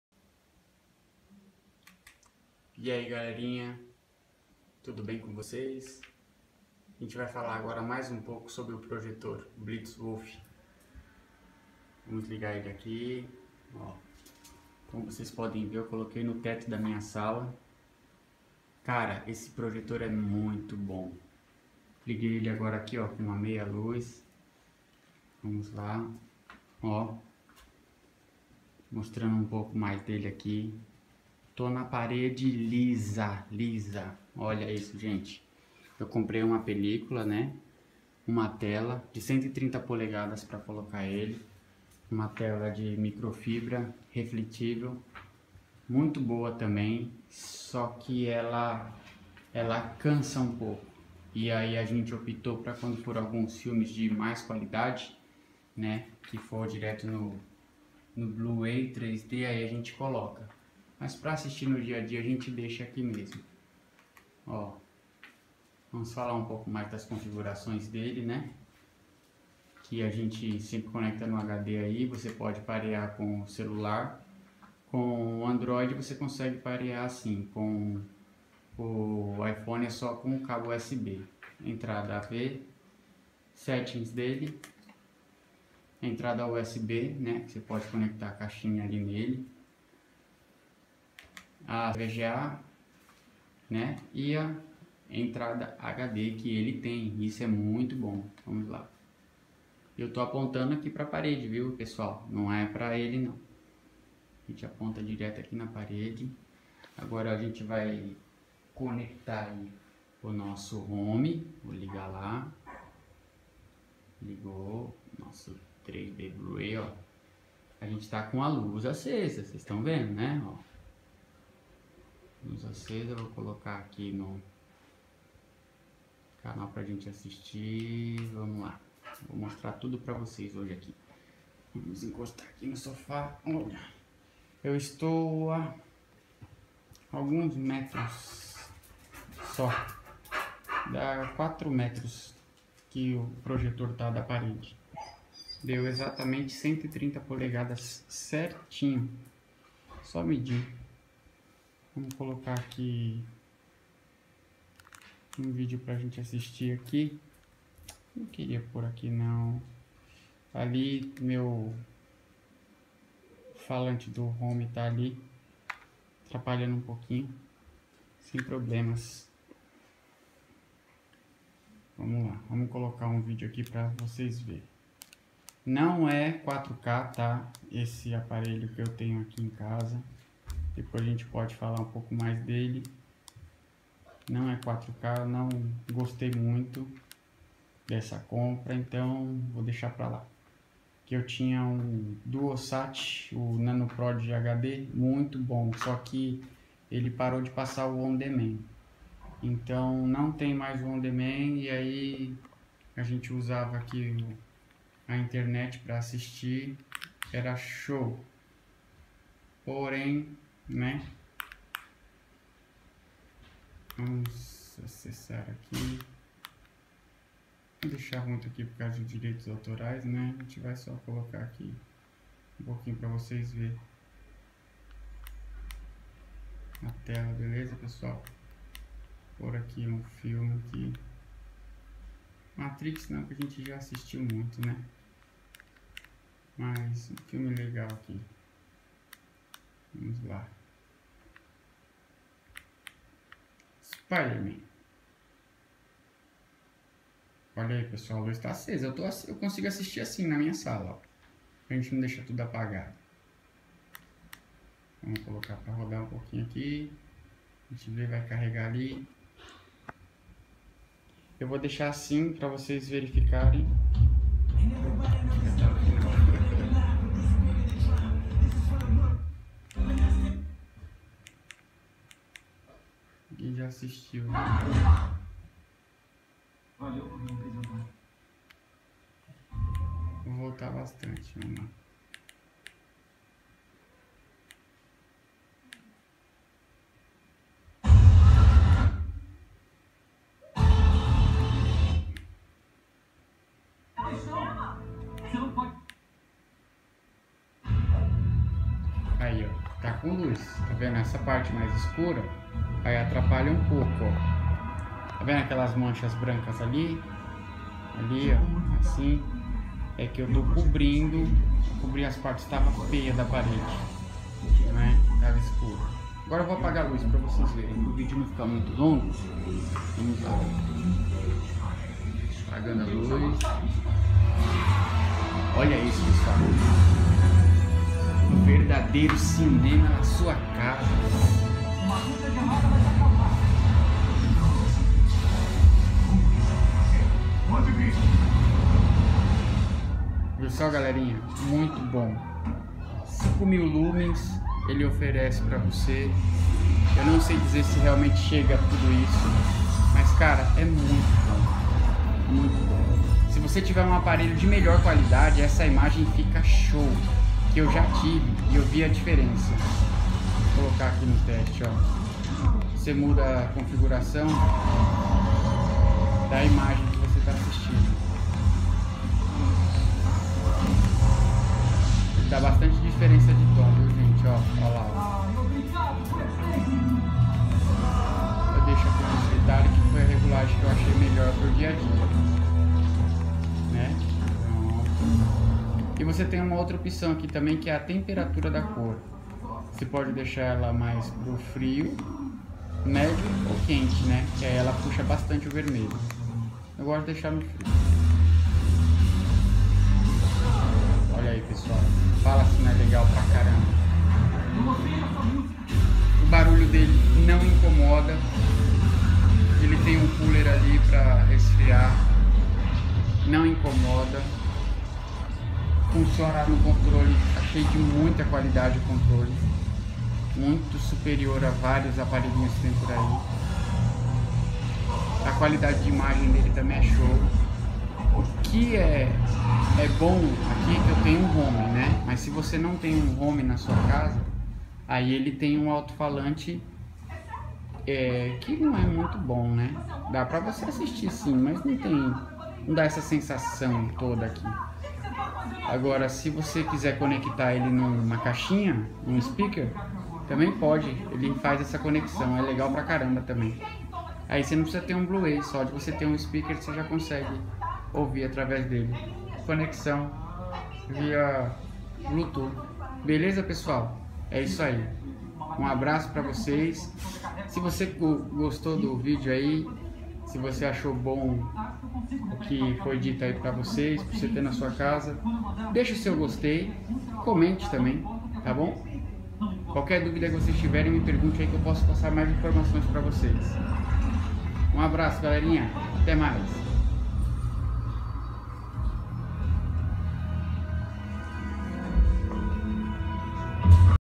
Qual melhor projetor ? excelente qualidade Eu recomendo.
Custo beneficio, qualidade e conectividade!

Importado pela Banggood
Cheguou com 35 dias !
Tive que pagar taxa aqui no Brasil ... 120,00
No site antes da pandemia por 800,00 .